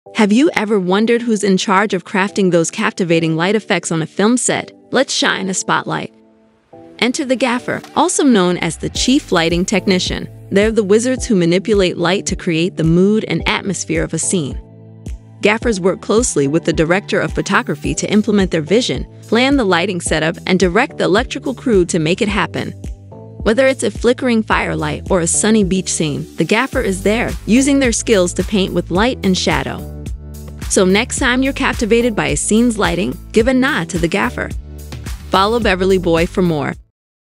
WHAT DOES A GAFFER DO?
Illuminate your understanding of film production with 'What Does a Gaffer Do?' Unearth the critical role of the Gaffer, a master of light who sculpts the scene's illumination, thereby setting the mood and aesthetics of the film. Walk alongside Team Beverly Boy as we spotlight this key figure, enhancing your knowledge of filmmaking, one luminary role at a time.